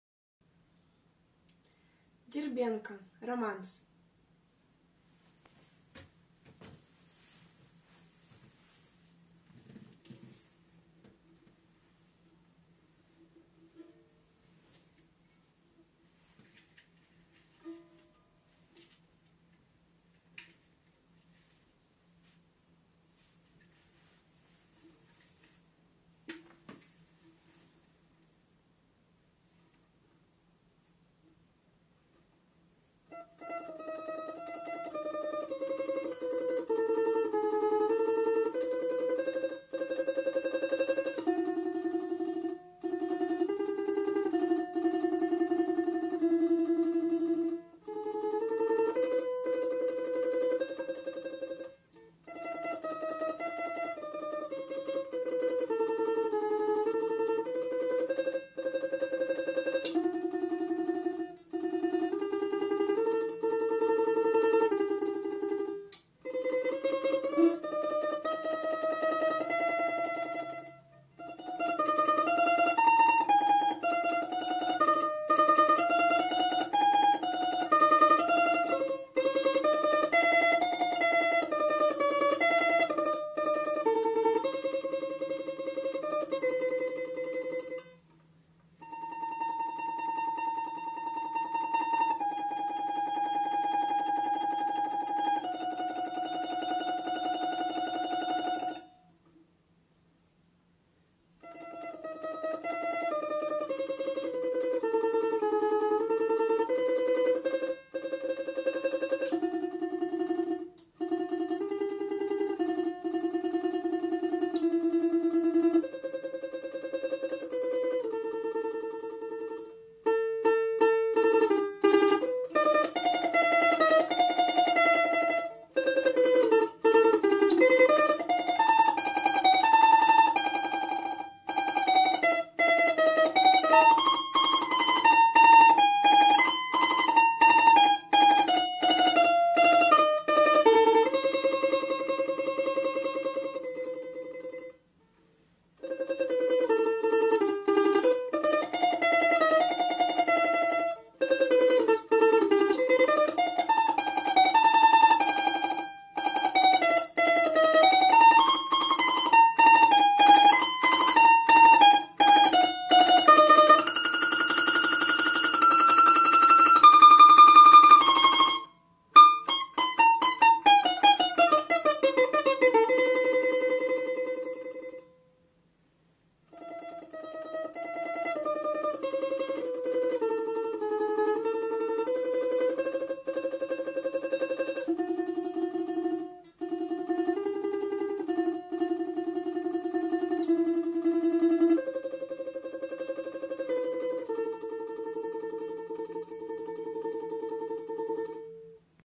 Олейникова Анна
Дистанционный отчетный концерт ДМШ Приморского краевого колледжа искусств в период карантина 2020 г.